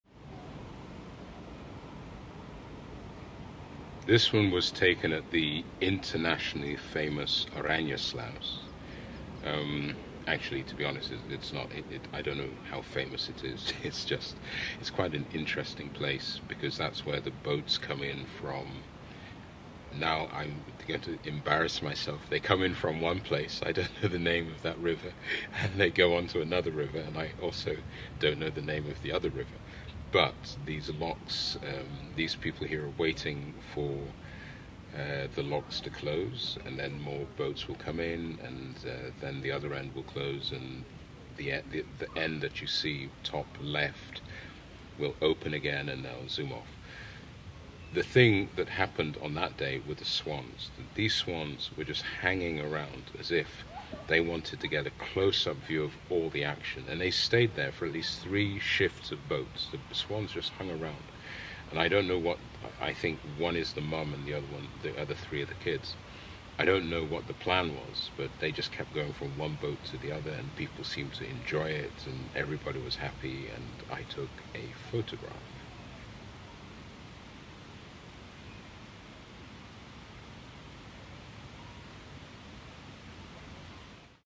Amsterdamica S05E11 – Swans At Oranjesluis
These swans have long since lost their fear of people or, perhaps they’ve worked out that people = an easy lunch – who knows… Whatever the case, it was interesting to watch the boats come in and out of this sluice, while the swans moved in and around the boats.